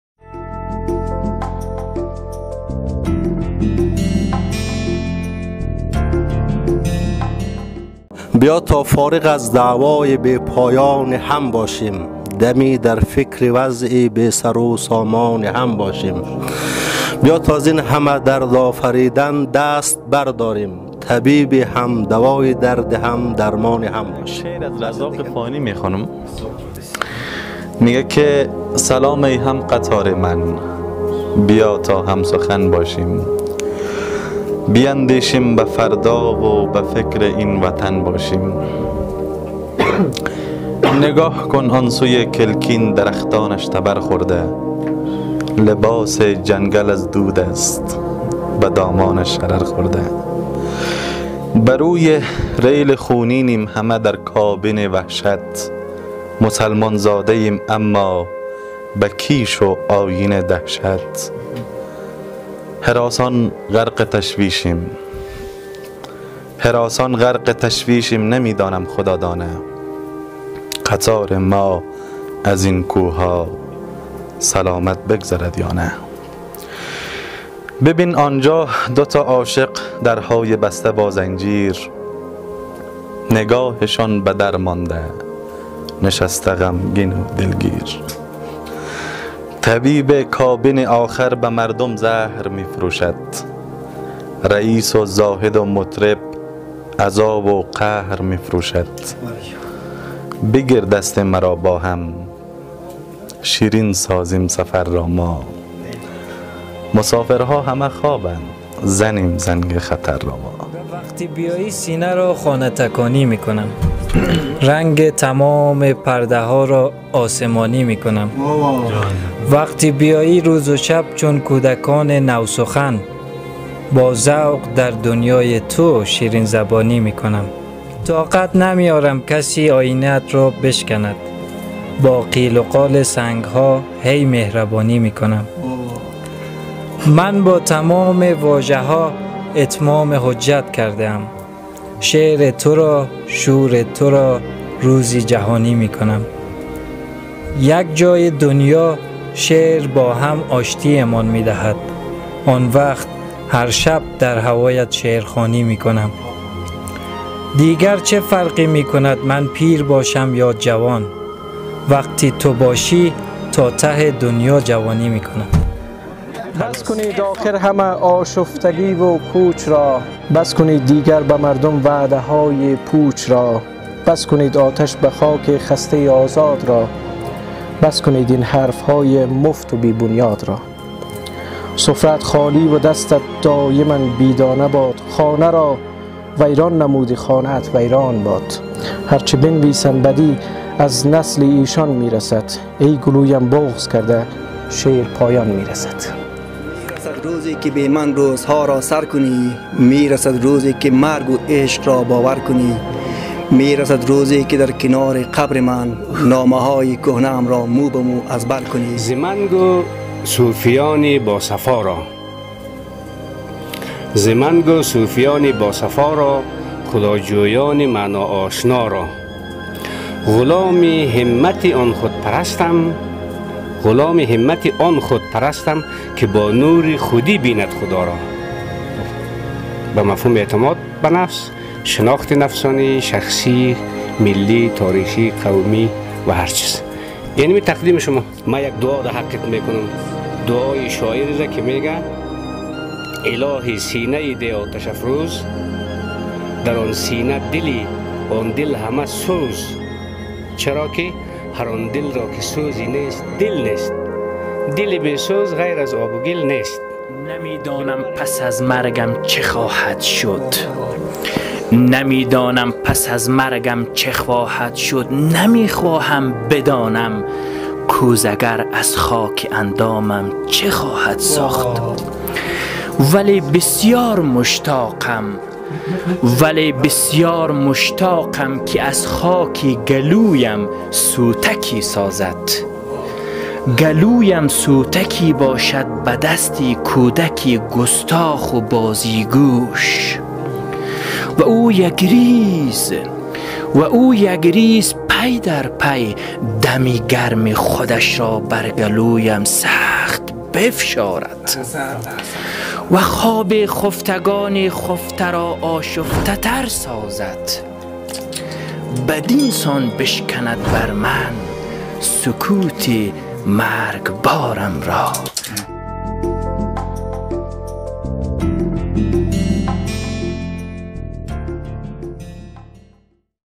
شب شعر/دوستانه/صمیمانه/ سرشار ازاحساسات وخاطره ساز
تلویزیون_آشیانه /دکلمه های زیبا وپراز احساس همکاران رسانه یی دریک شب به یادماندنی کابل